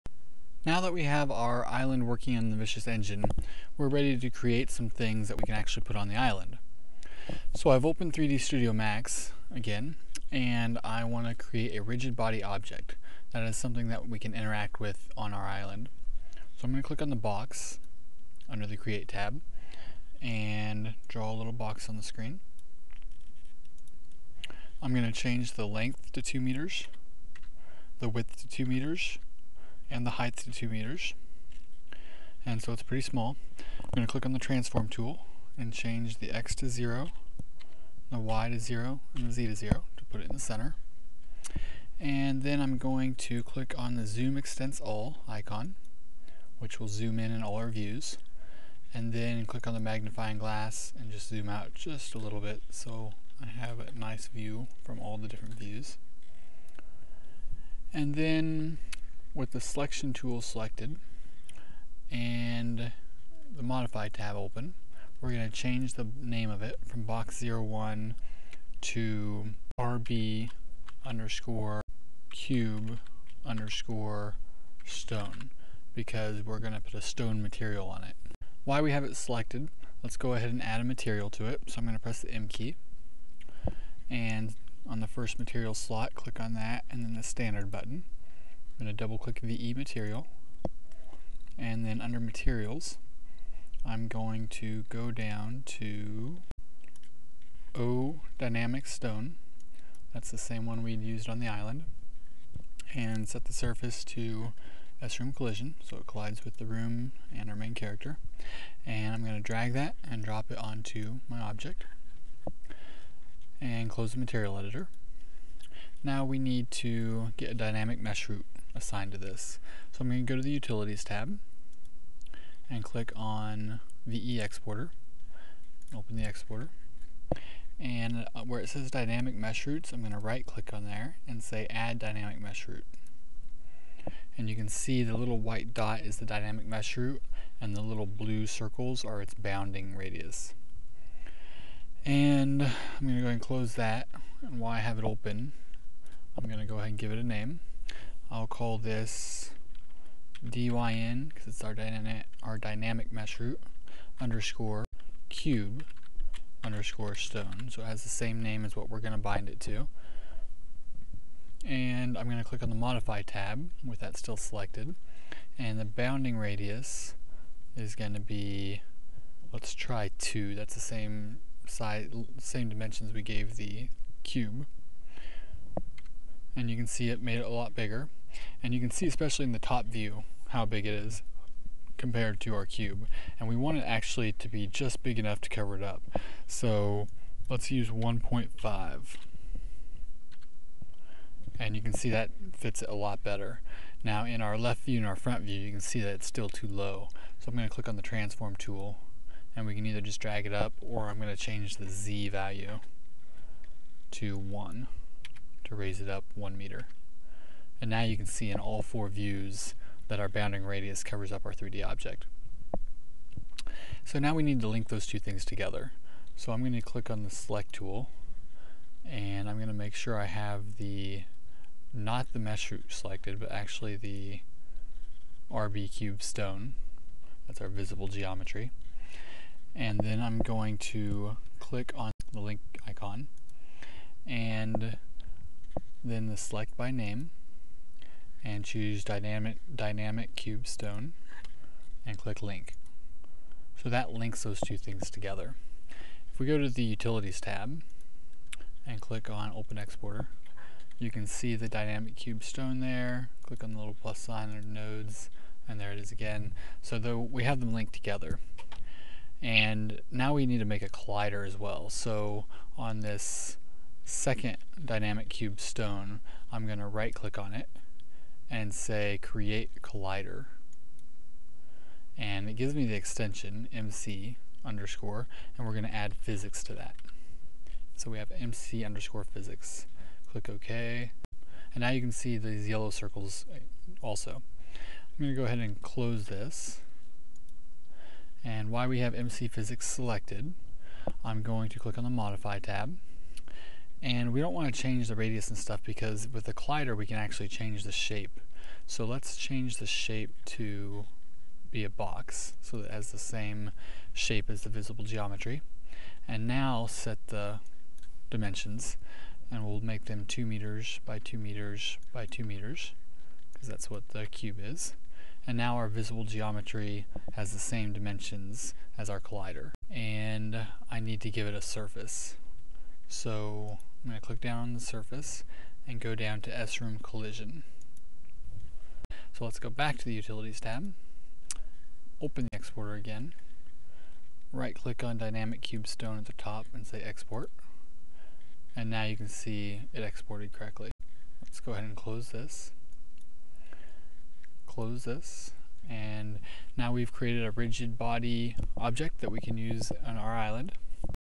Lesson 14: Create a Rigid Body Object
In this tutorial, we will create a rigid body object that can be manipulated by the game character.

All videos tutorials as well as a printable document of the steps for each video are available